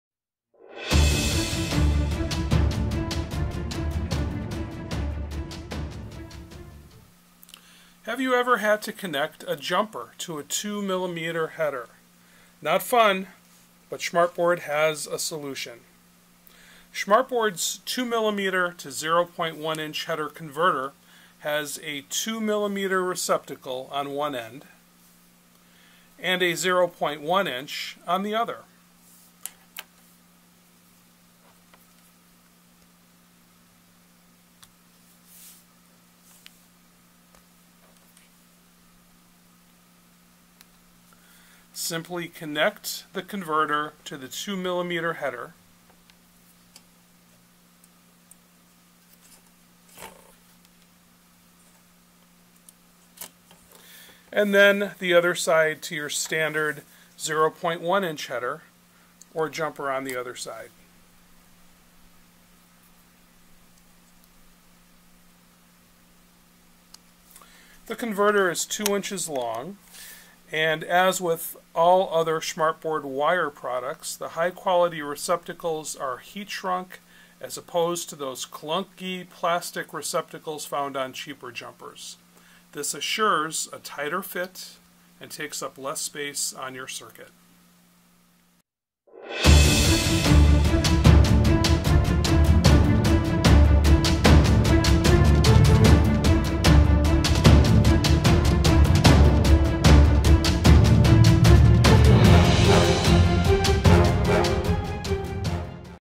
Schmartboard Qty. 10 2mm to 0.1 Inch Header Converter
2mm header posts are not easy to work with.  You can't find jumpers with a snug 2mm receptacle.  Until now you were stuck with cheap loose fitting 2mm receptacles or expensive custom solutions.  The Schmartboard 2mm to 0.1" converter allows you to use standard 0.1 inch jumpers with a 2mm post.  Simply slip the 2mm side onto the 2mm post and connect a standard .1 inch male header to the other end of the converter.  Our receptacles are tight.  This is important because you certainly do not want to have a jumper wire as the culprit in a problematic circuit.  We made these due to numerous customer requests for a better solution.